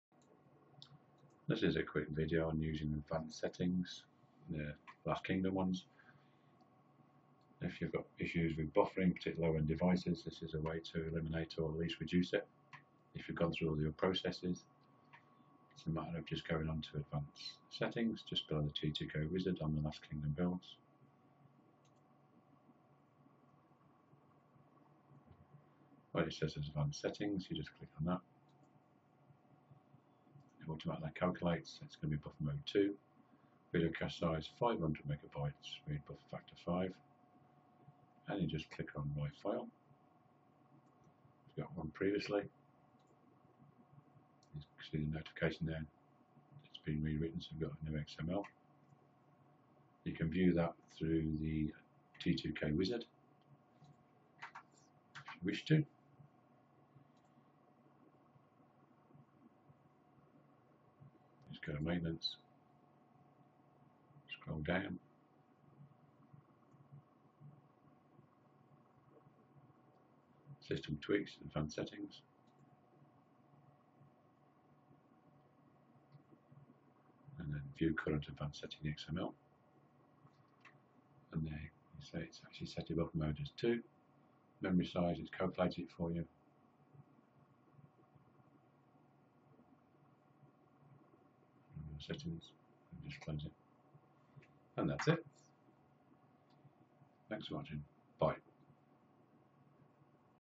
Buffering issues Using last kingdom Adanced Settings
just a couple of clicks to auto calculate the settings for your device